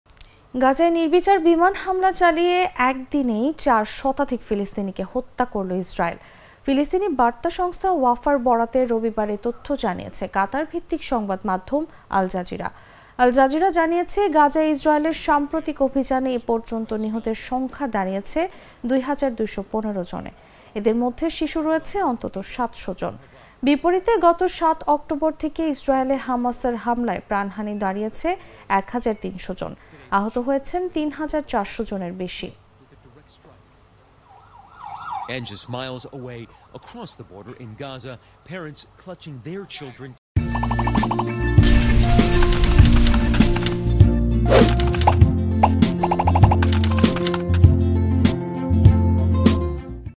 একদিনেই ৪০০ ফিলিস্তিনিকে হ'ত্যা করলো ইসরায়েল, মোট ২২১৫ | Israel | Palestine || Jago News
একদিনেই ৪০০ ফিলিস্তিনিকে হ'ত্যা করলো ইসরায়েল, মোট ২২১৫

গাজায় নির্বিচার বিমান হামলা চালিয়ে একদিনেই চার শতাধিক ফিলিস্তিনিকে হত্যা করলো ইসরায়েল। ফিলিস্তিনি বার্তা সংস্থা ওয়াফার বরাতে রোববার (১৫ অক্টোবর) এ তথ্য জানিয়েছে কাতারভিত্তিক সংবাদমাধ্যম আল-জাজিরা।

খবরে বলা হয়েছে, গত ২৪ ঘণ্টায় ইসরায়েলি হামলায় গাজা সিটিতে ২৬০ জন, উপত্যকার মধ্যাঞ্চলীয় দেইর আল-বালাহতে ৮০ জন এবং উত্তরাঞ্চলীয় জাবালেয়া শরণার্থী শিবিরে প্রাণ হারিয়েছেন অন্তত ৪০ জন ফিলিস্তিনি। এছাড়া বেইত লাহিয়া শহরে ১০ জন এবং দক্ষিণাঞ্চলীয় খান ইউনিসে নিহত হয়েছেন আরও ২০ জন।

আল-জাজিরা জানিয়েছে, গাজায় ইসরায়েলের সাম্প্রতিক অভিযানে এ পর্যন্ত নিহতের সংখ্যা দাঁড়িয়েছে ২ হাজার ২১৫ জনে। এদের মধ্যে শিশু রয়েছে অন্তত ৭০০ জন। হামলায় আহত হয়েছেন আরও ৮ হাজার ৭১৪ ফিলিস্তিনি।

এছাড়া, গত সপ্তাহ থেকে পশ্চিম তীরে ইসরায়েলি হামলায় নিহতের সংখ্যা অর্ধশত ছাড়িয়েছে।

বিপরীতে, গত ৭ অক্টোবর থেকে ইসরায়েলে হামাসের হামলায় প্রাণহানি দাঁড়িয়েছে ১ হাজার ৩০০ জন। আহত হয়েছেন ৩ হাজার ৪০০ জনের বেশি।

শনিবার গাজায় রাতভর বোমাবর্ষণ করেছে ইসরায়েলি বাহিনী। এছাড়া, খান ইউনিসে স্থল অভিযানও চালিয়েছে তারা। জবাবে তেল-আবিবের দিকে রকেট ছুড়েছে হামাস।